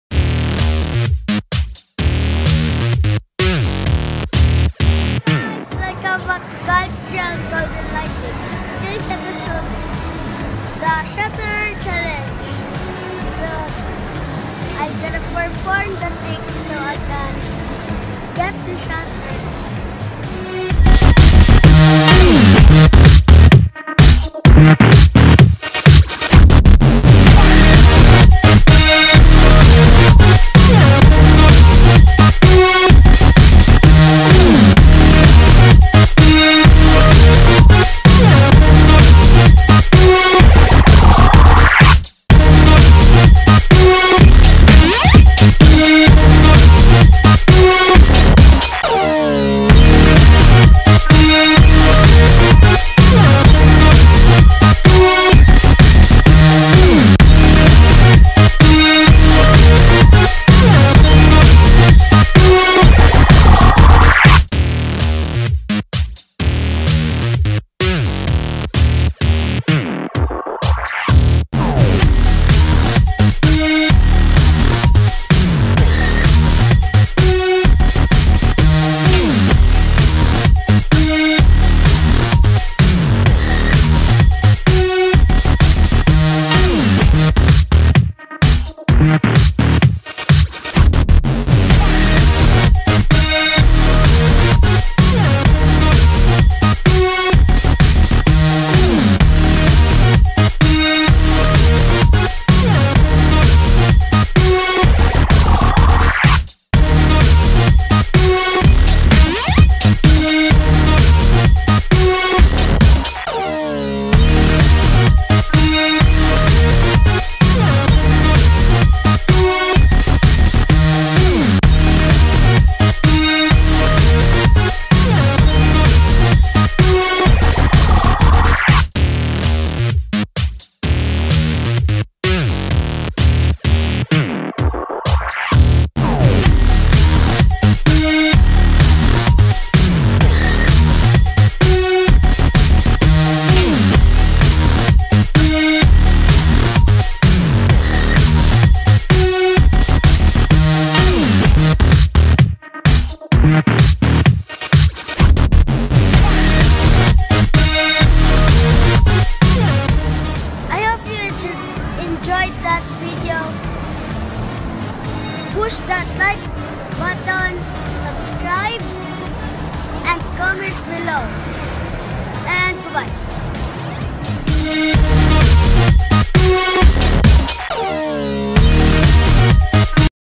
Kyle TGL Shutter Challenge
Just a little something fun to get me a YYF Shutter.  Had fun doing make the vlog, hope you had fun watching it.